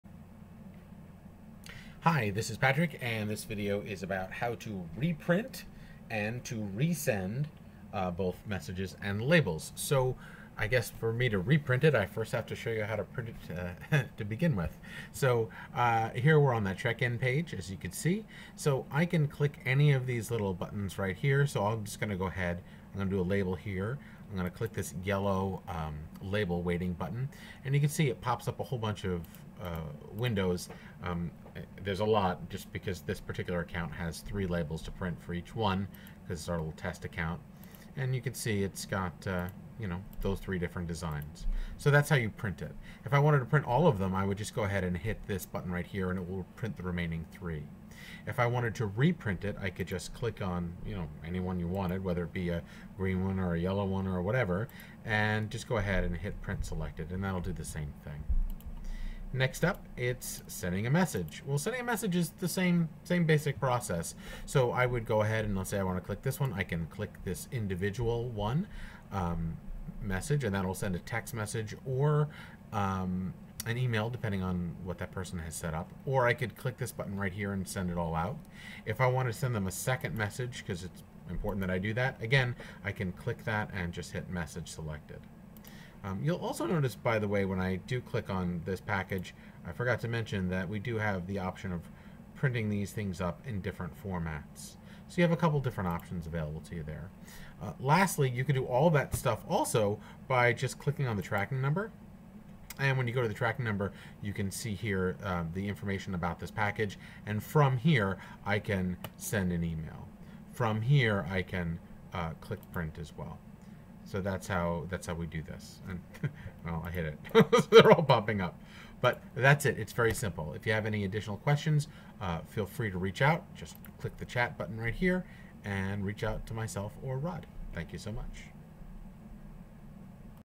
Reprinting and Resending
This video shows you several ways to reprint labels and resend messages within the EZTrackIt system.